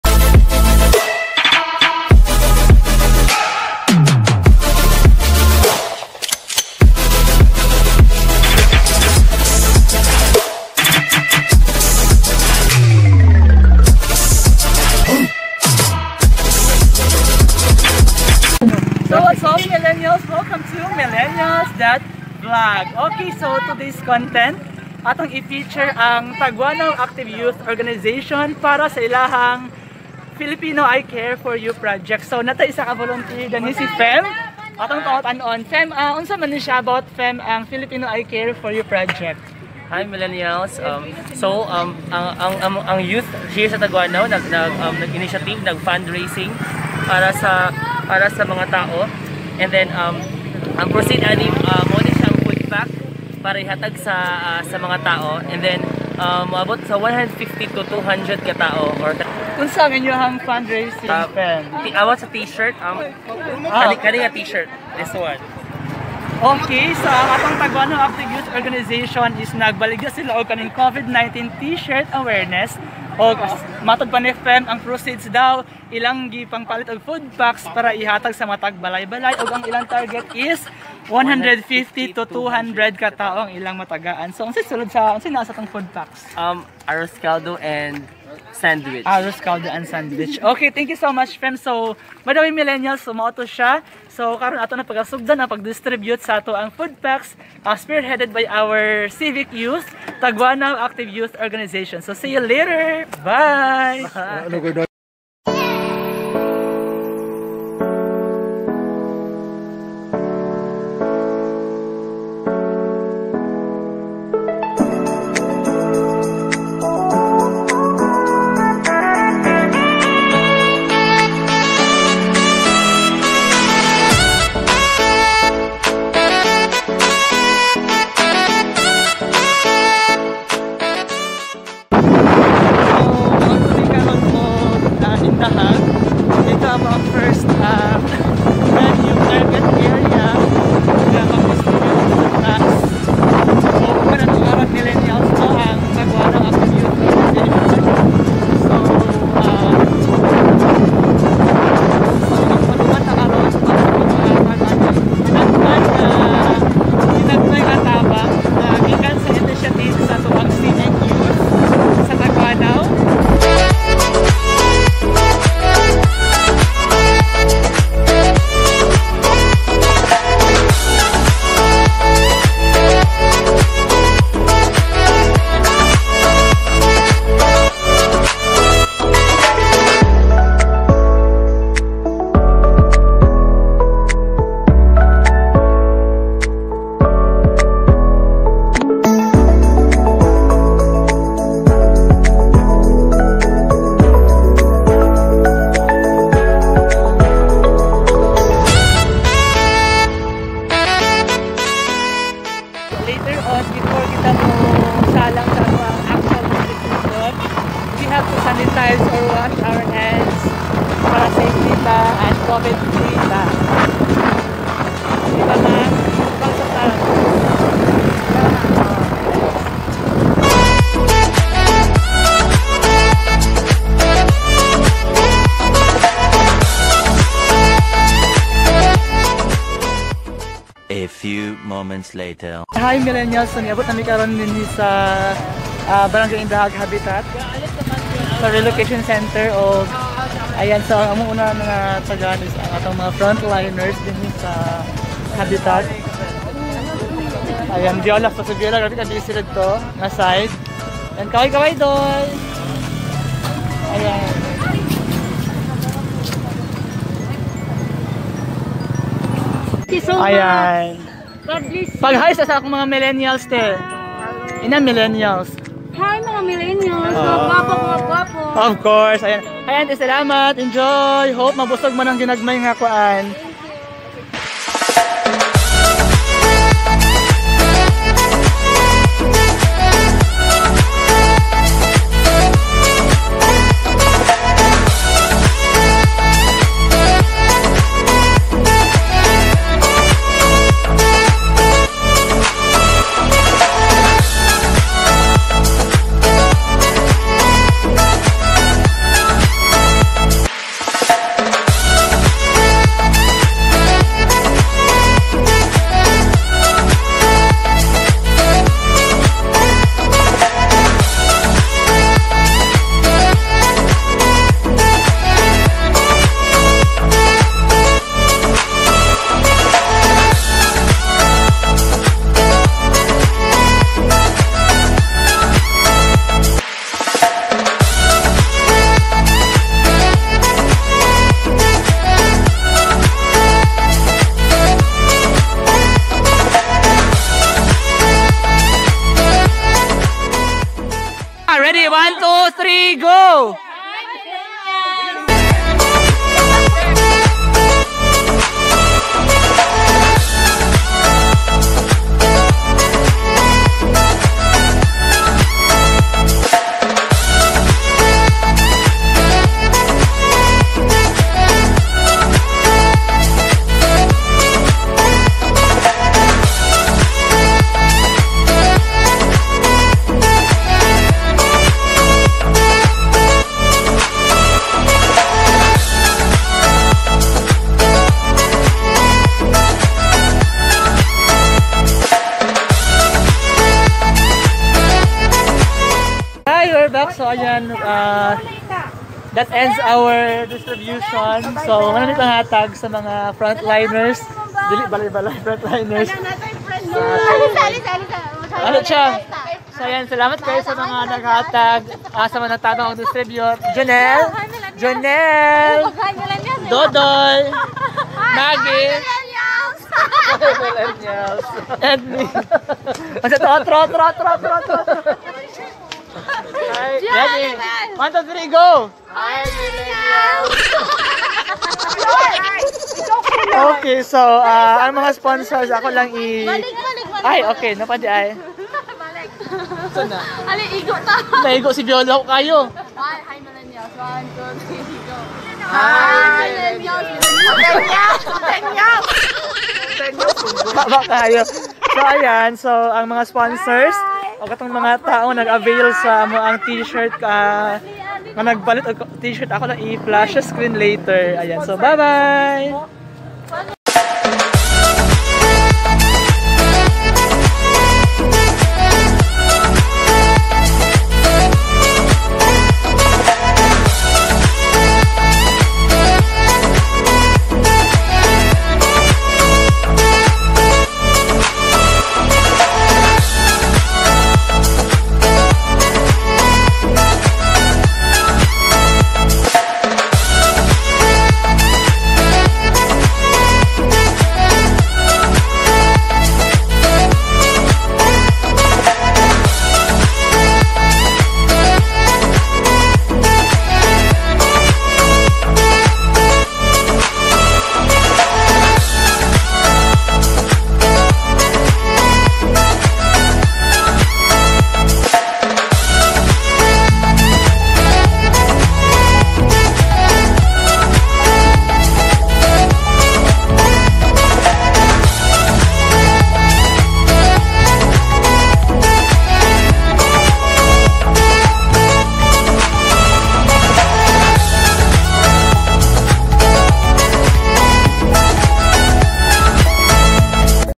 Pilipino: I Care for U | Taguanao Active Youth Organization | A little help for our Frontliners
Taguanao Active Youth Organization (TAYO) is a group of young people from Taguanao Indahag Cagayan de Oro City who initiated the "Pilipino: I Care for U" project. A fund raising project through selling a COVID-19 Awareness Tshirt that aims to help and give food packs to our dear frontliners.